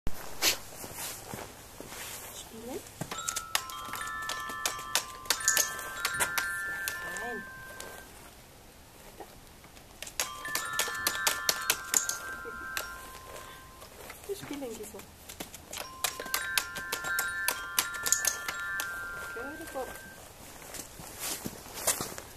Gizmo haut in die Tasten
Ich glaub, Gizmo bekommt zu Weihnachten ein großes Keyboard.
So ein Talent muß man fördern...